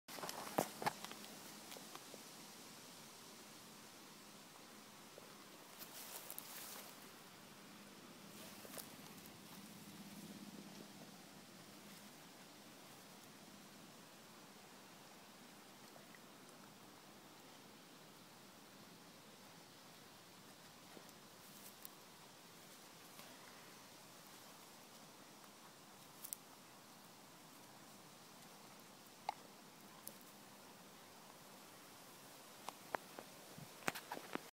Raspberry Redi
On a walk around the block Redi suddenly stopped and started smelling some bushes. And then, as if we're not feeding him enough, he started eating raspberries! :-D